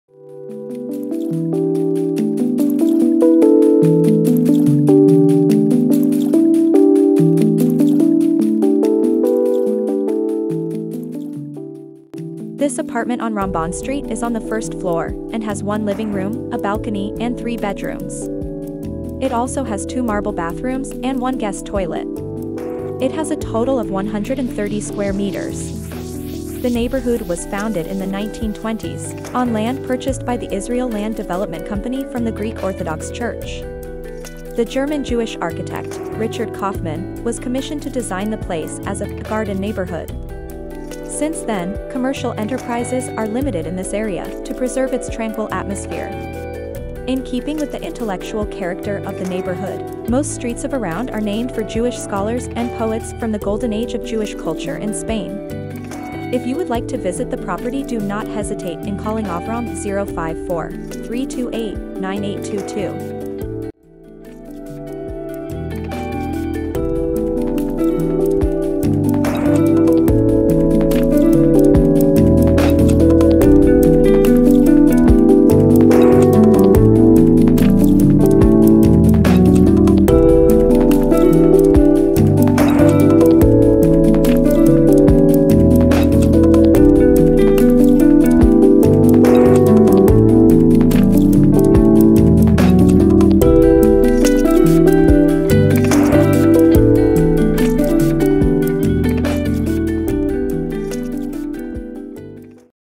ramban 17
130 square meters
1 lving room
3 bedrooms
2 marble bathrooms
1 guest toilet
first floor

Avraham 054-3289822
Moshe 46285378